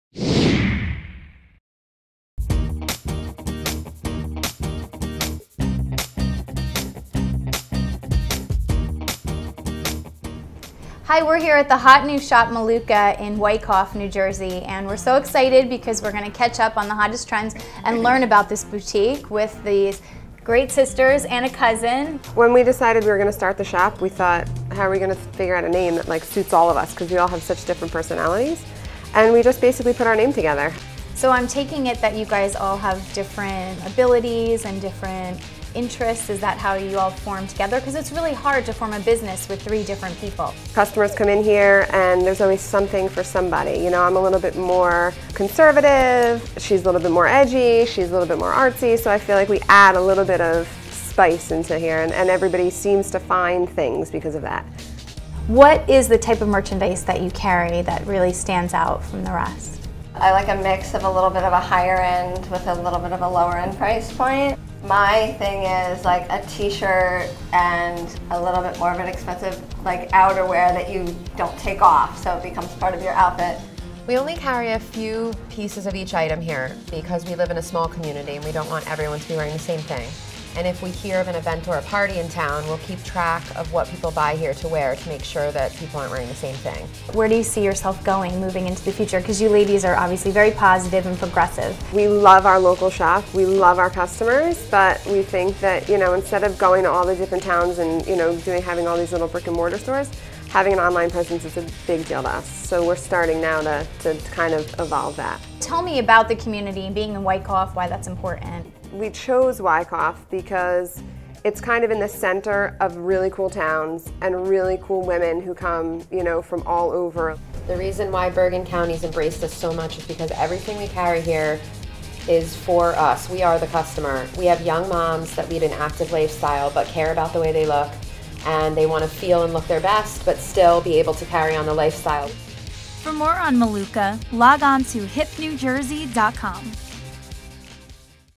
Hip New Jersey: Maluka Boutique
Would you say your style is "edgy" or "artisic"? Or do you appreciate a more safe and confortable look? Regardless, at Maluka, there is a guaranteed fashion find for all.

Maluka is a Jersey luxe, family owned fashion boutique located in the heart of Bergen County in Wyckoff, NJ.  Similarly to the name of the store, all inventory within the boutique reflects a little bit of each owners' personality. HipNJ's Gina La Morte visited the boutique, where she spoke with store owning trio, Marcy, Lulu (Lindsay), and Kari.

Since the store opened its doors in 2006, it has become the go-to fashion place in suburbia. What makes Maluka stand out amongst other stores in the area is their patent sense of small town and community. (The store carries only a few pieces of each hand-picked item from their collections-- so there's no wardrobe repeats at that fab dinner party!)  This simple strategy keeps their business flourishing, because their customers receive an individualized look a part from anyone else.

When you shop at Maluka, whether online or in the boutique, it is their top priority to make sure that you are getting the best the industry has to offer. They want to make sure that you, the customer, have as much fun shopping for yourselves as they have shopping for you!